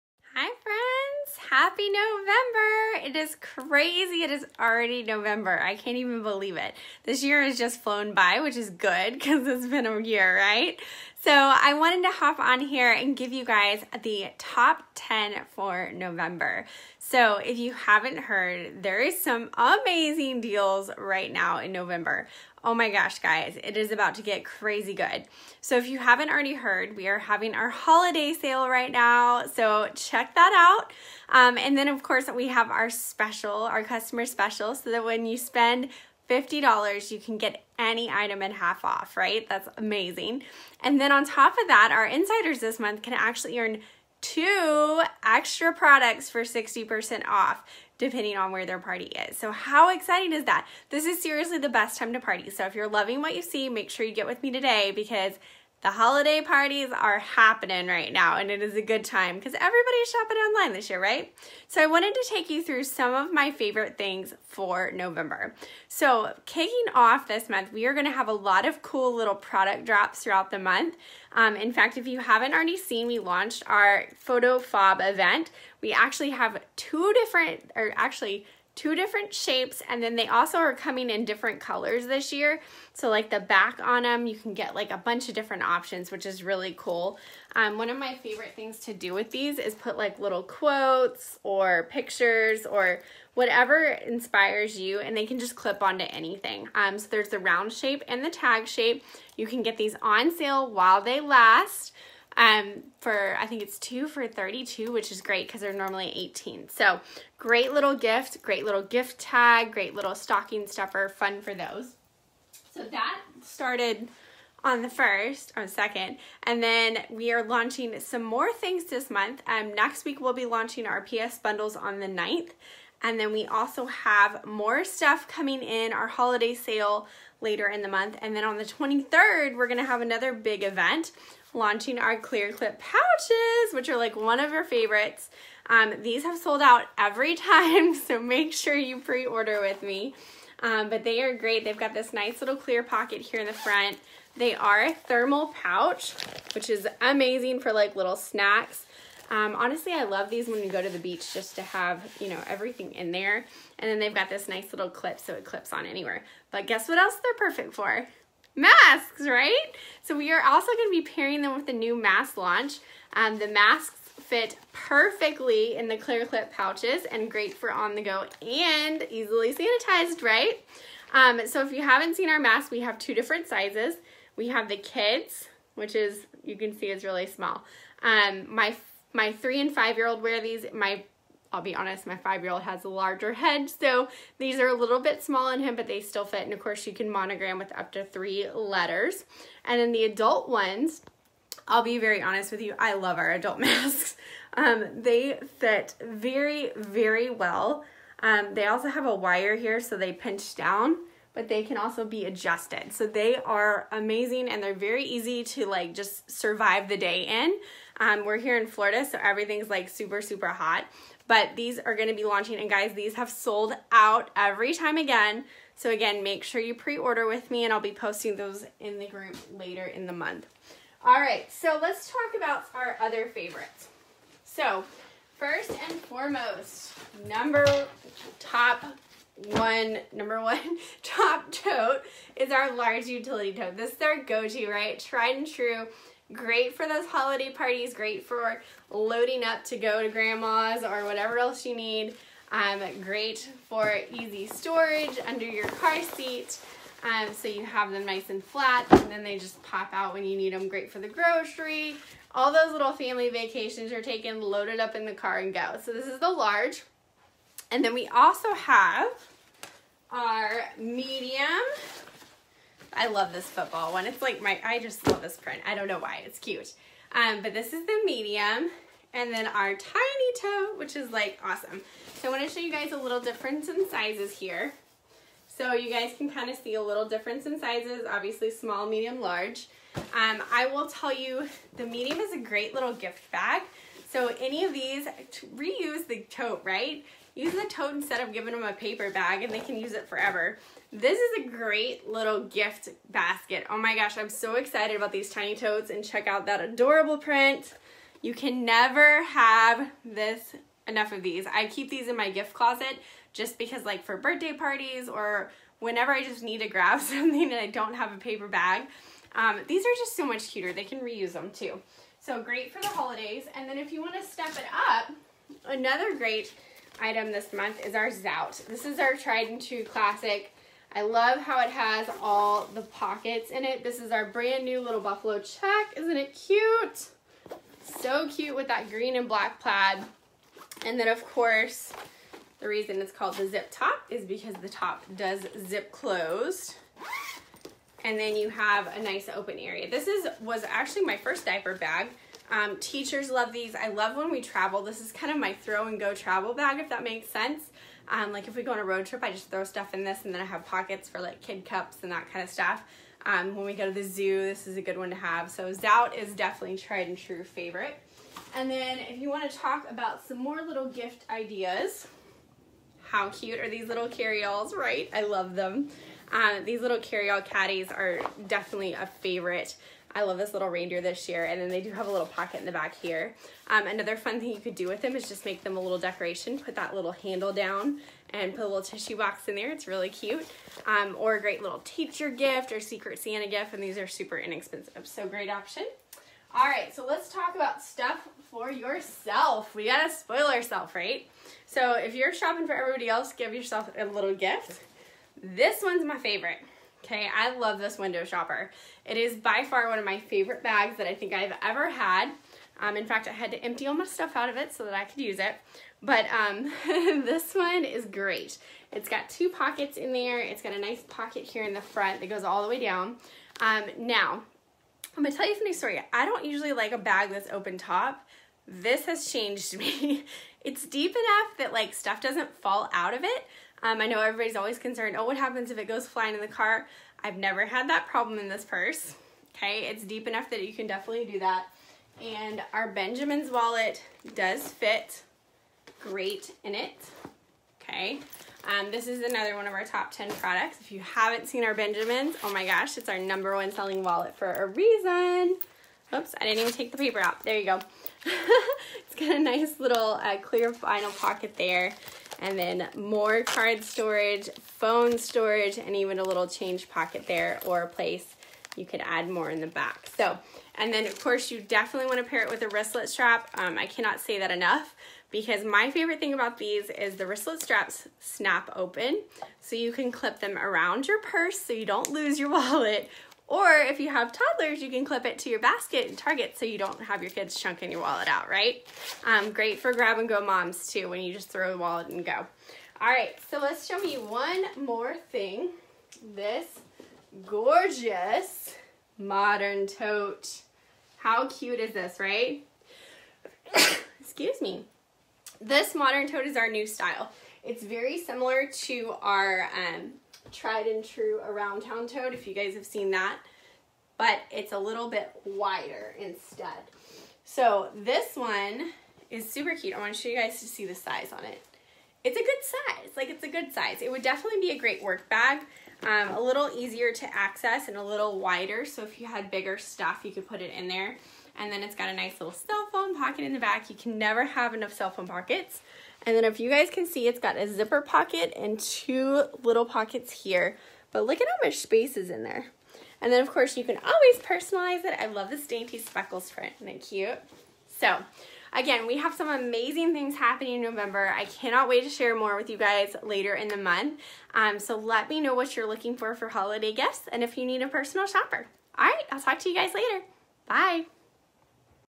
November Top 10 🔥HOT🔥List
1) Clear Clip pouch with Face Masks
2) Phot Key fobs
3) Large Utility Tote
4) All About the Benjamins Wallet with Wristlet Strap
5) Modern Tote
6) Window Shopper
7) Tiny Tote
8) Medium Utility Tote
9) Littles Carry All Caddy
10) Zip Top Organizing Utility Tote

Order on my ELITE monthly giveaway to be entered in to win